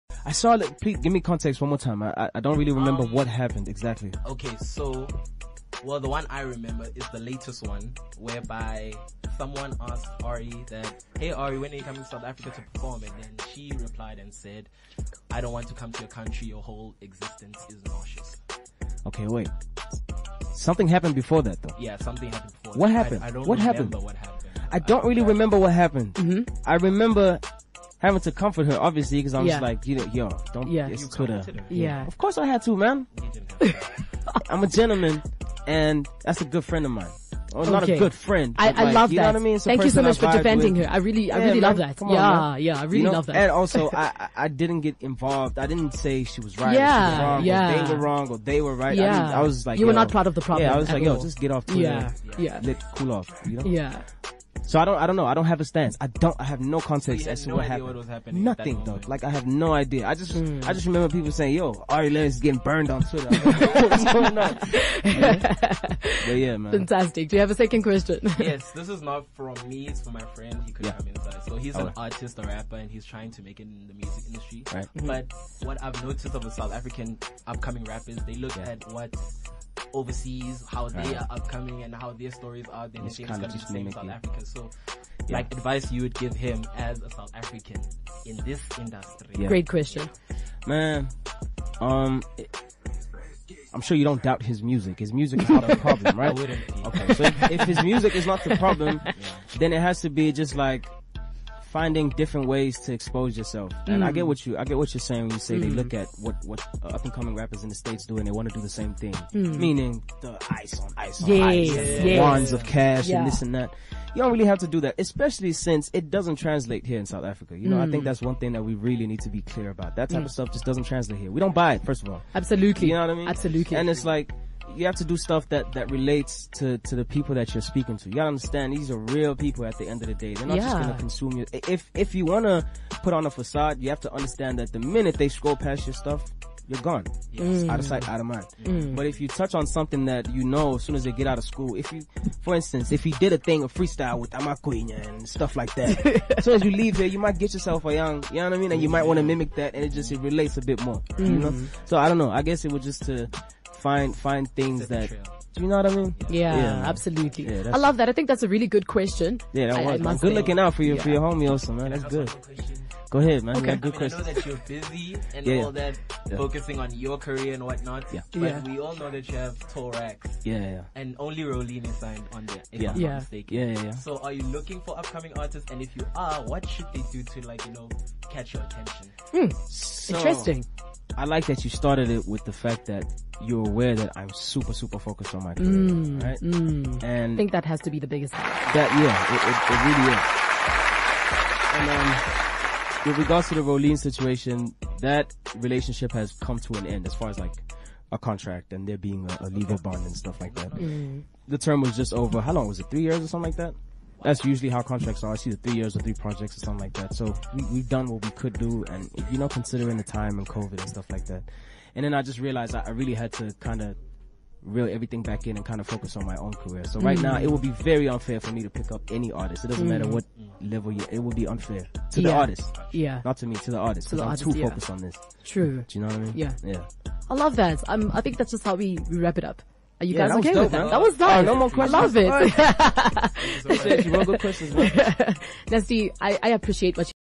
Nasty C on Ari Lennox & MacG situation, Rowlene no longer under Tall Racks & Signing Artists
Nasty C talks about Mac G and Ari Lennox situation, Rowlene going independent and if he’ll ever sign new artists to Tall Racks Records